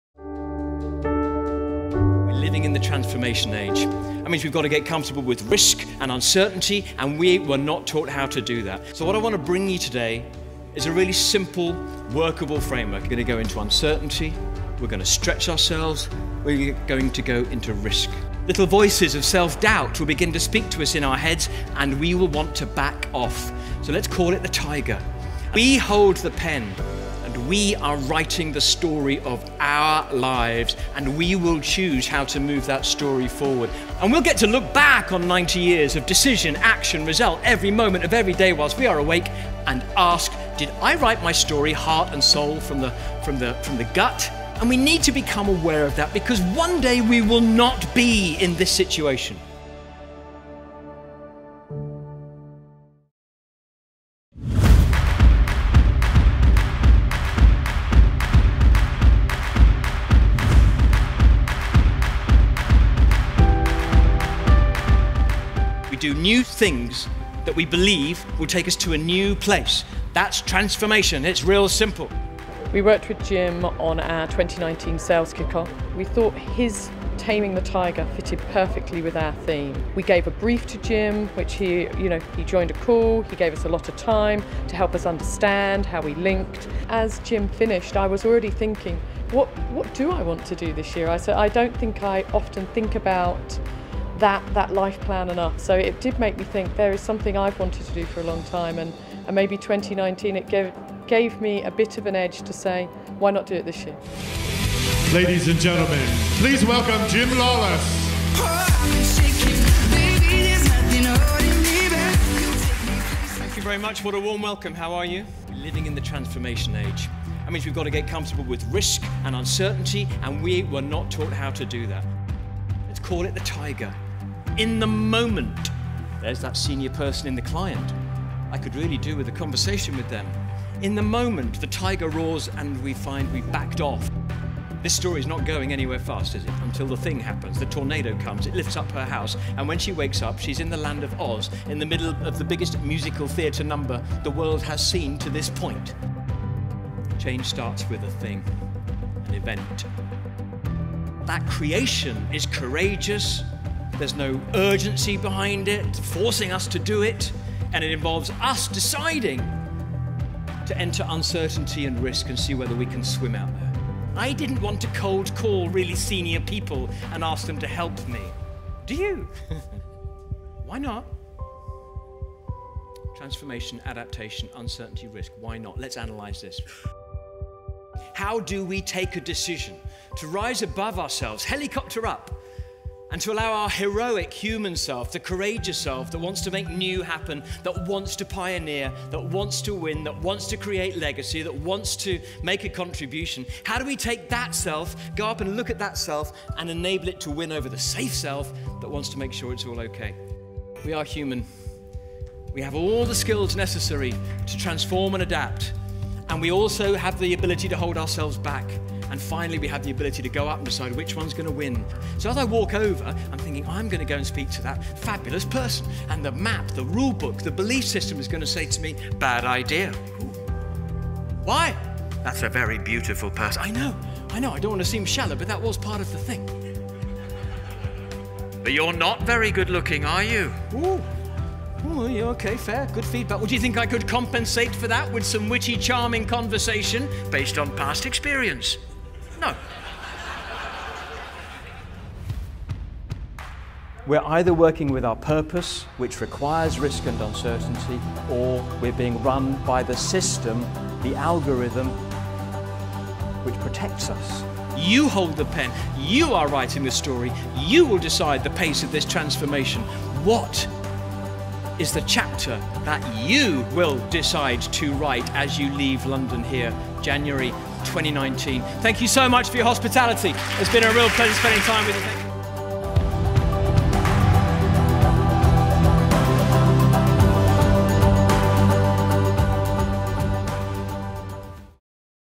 Jim Lawless - How do we change, adapt, transform to keep up in the digital age
Jim Lawless -Understanding what makes change tough, Taming Tigers: Accomplishing "Mission Impossible"